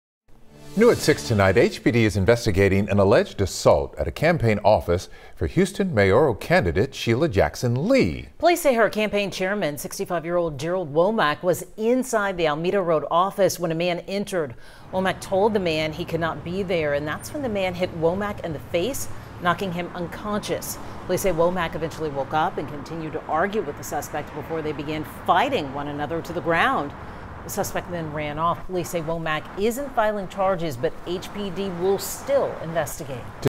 Sheila Jackson Lee's campaign chairman knocked out in brawl at Houston campaign satellite office
The fight apparently started when Gerald Womack told the man he couldn't be inside the office.